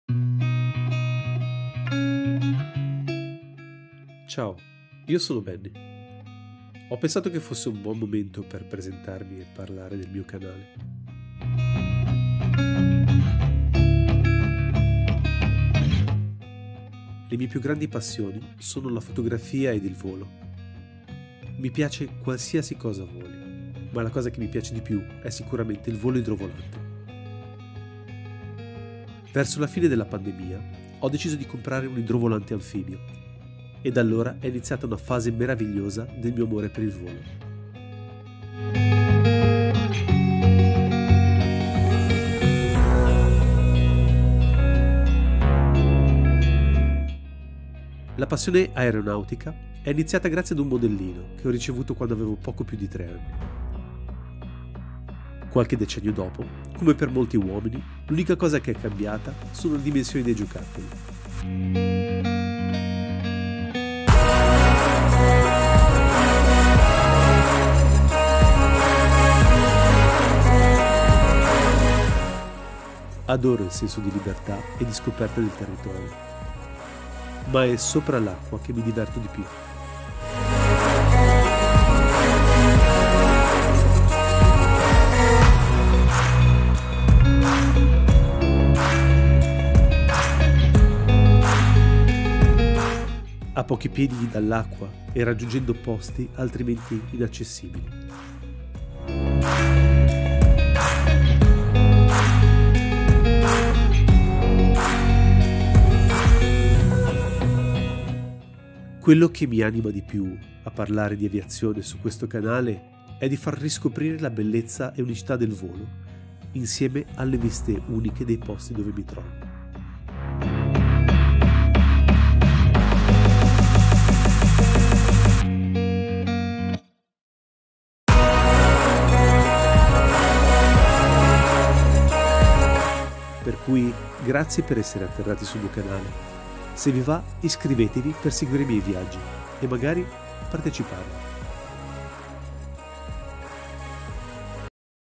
I miei viaggi in idrovolante
Benvenuti in questo primo video del mio canale in Italiano.
Mi presento brevemente e vi parlo di I-76, il mio idrovolante anfibio.

0:00 Chi sono
0:13 Le mie passioni
0:28 Il mio idrovolante
0:46 Com'è nato l'amore per il volo
0:50 Volare oggi
1:22 Volare sull'acqua
1:44 Perché fare un canale YouTube
2:08 Conclusione